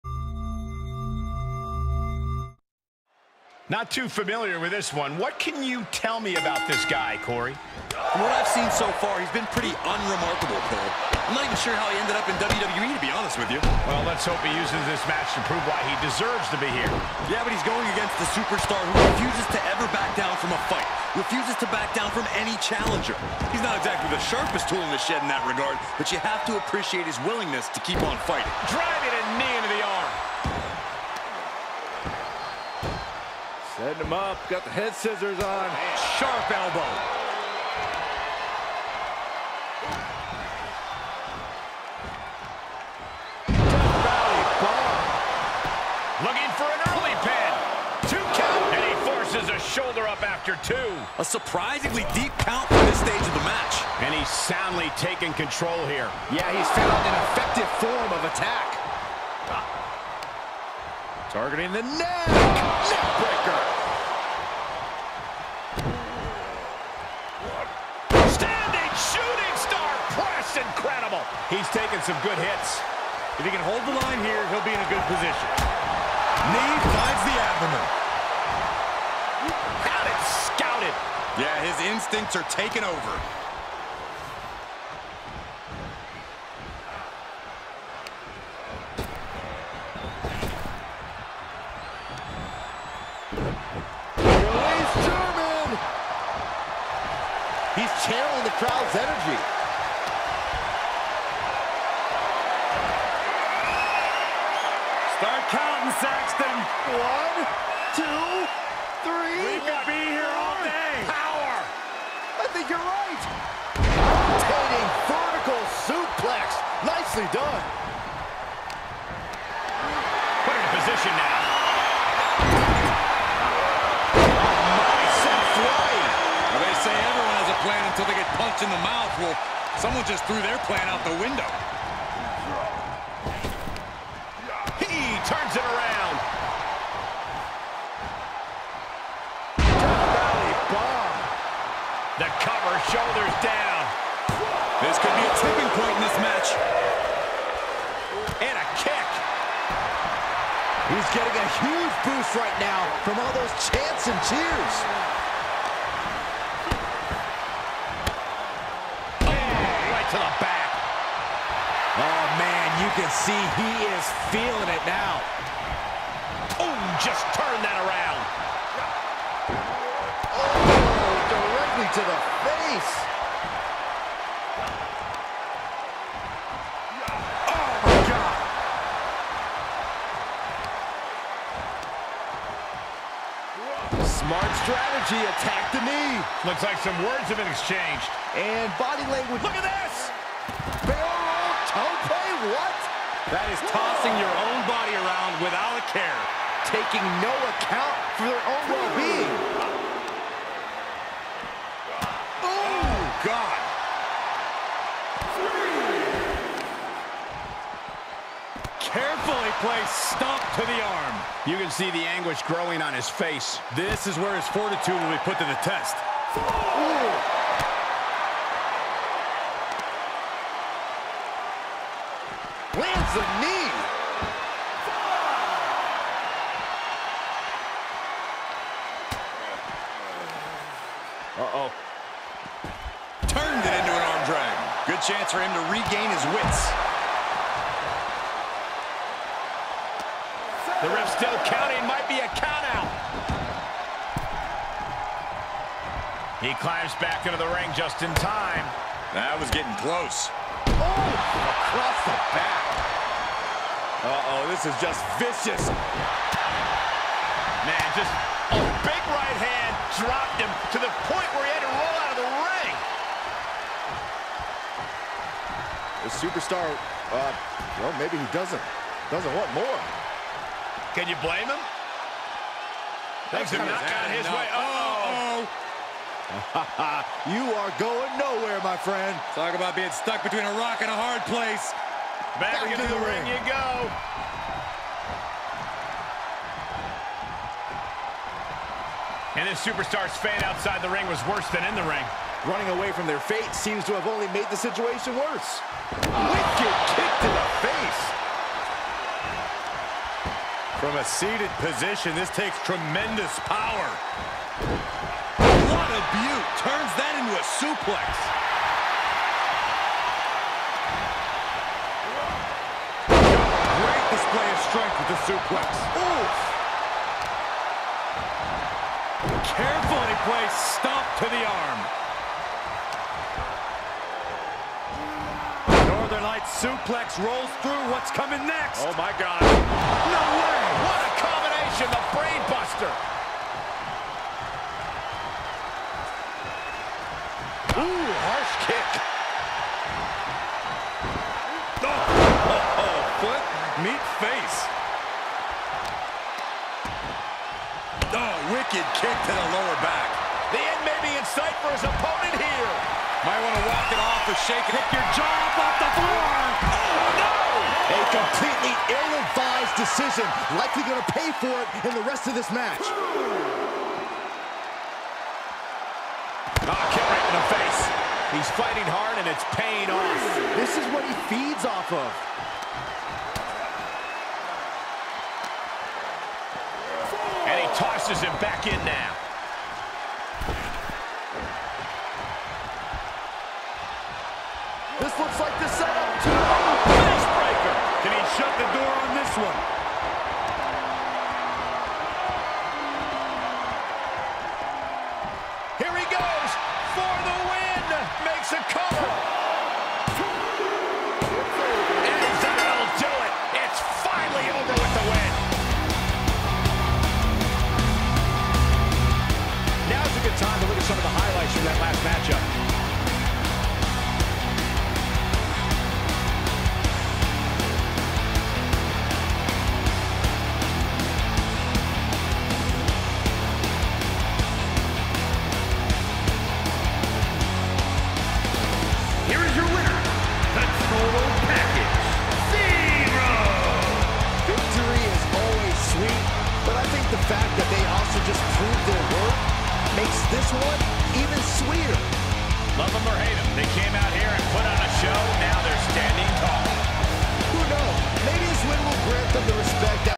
TPJW - Patina Ep19 - Pt 2
Zero VS Vulgus - Vulgus VI looks to capture some singles success. Total Zero doesn't think that will happen.